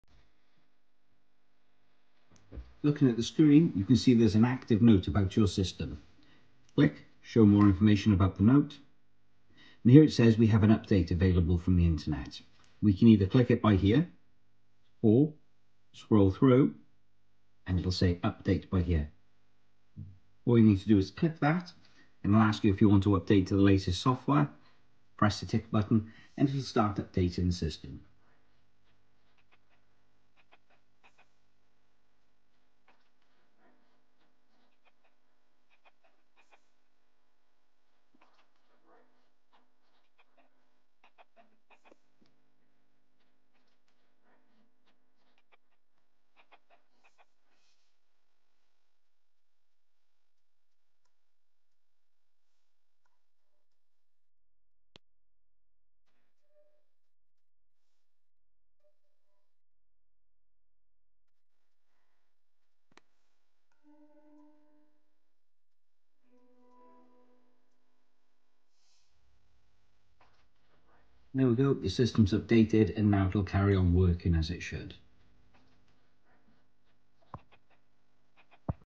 NIBE S Series - Software update via Wi-Fi
Software can be updated in 2 different ways first via the Wi-Fi if you have your unit connected to your home Wi-Fi. if you do not have Wi-Fi or unit connected you can still update your software, but will have to be done via the USB port. Simply go to the NIBE website and download the software to PC/Laptop and copy to the USB stick, then you need to insert it to the SMO via the USB port and it will guide you through the process. This will be covered on another video

*adjusting the system without fully understanding it may cause adverse effects such as increased running costs or damage to the system. If in doubt always refer to the user manual or seek expert advise.